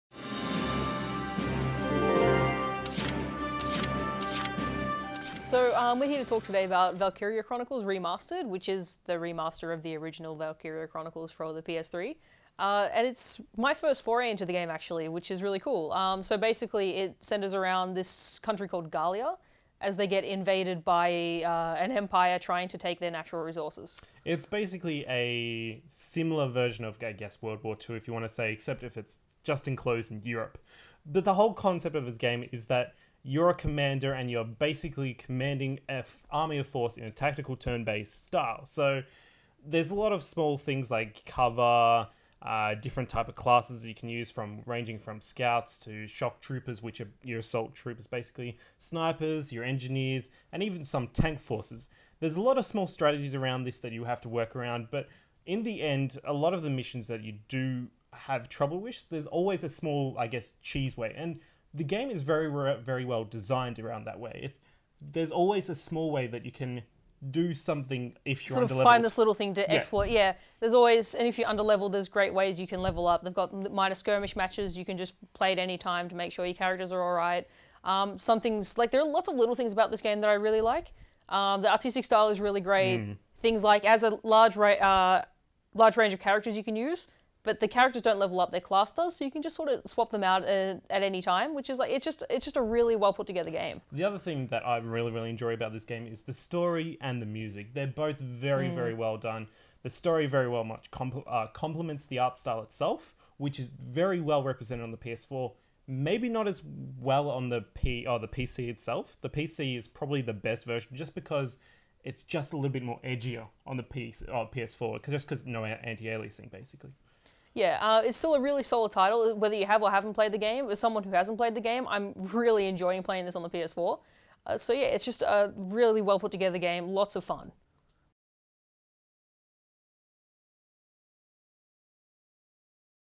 Valkyria Chronicles Remastered - Review
Shane and Charlotte revisit one of the more underrated RPGs of the last generation, now with a fresh coat of paint! Does Valkyria Chronicles hold up in 2016?